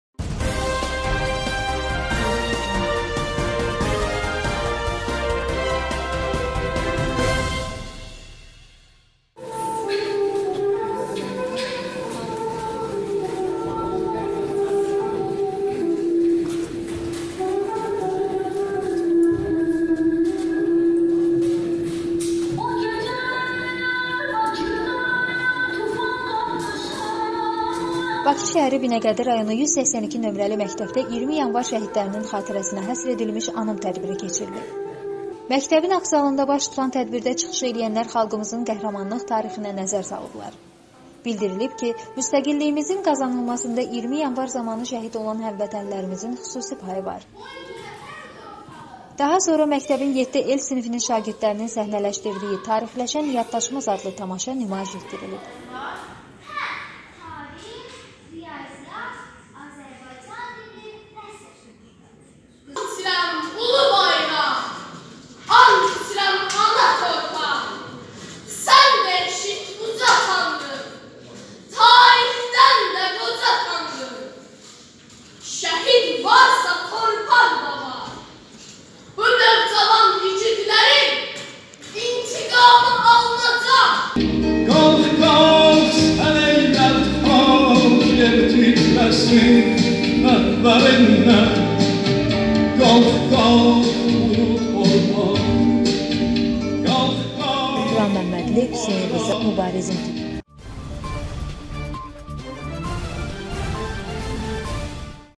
Şagirdlər şəhidlər haqqında tamaşa hazırladı
182 nömrəli məktəbdə “20 yanvar” şəhidlərinin xatirə tədbiri